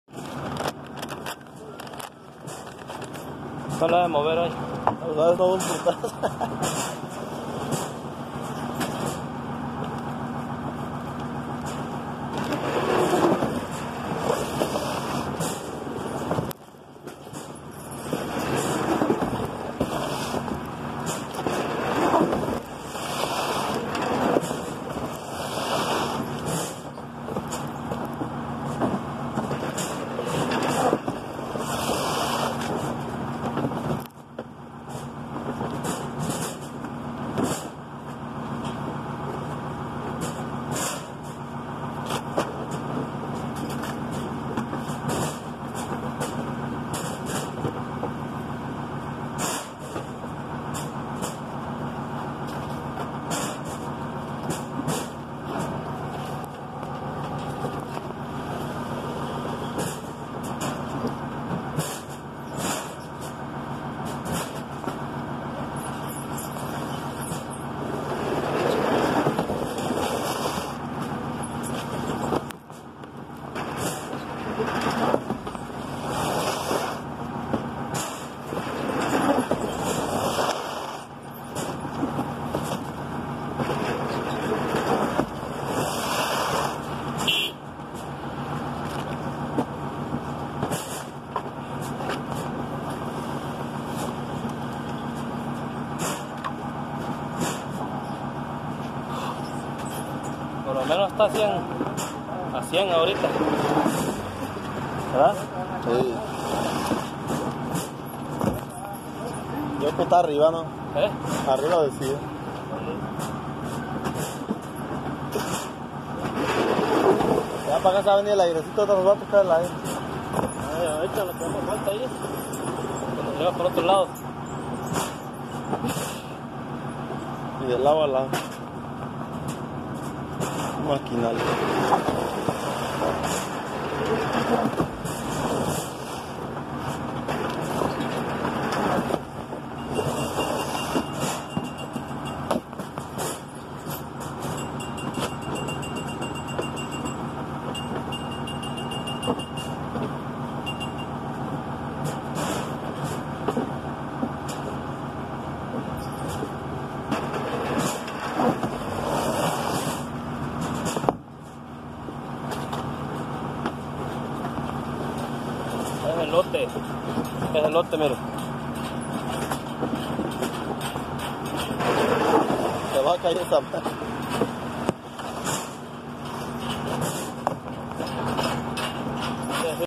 Mendota California cargando elotes
Bajo una gran calor cargando elotes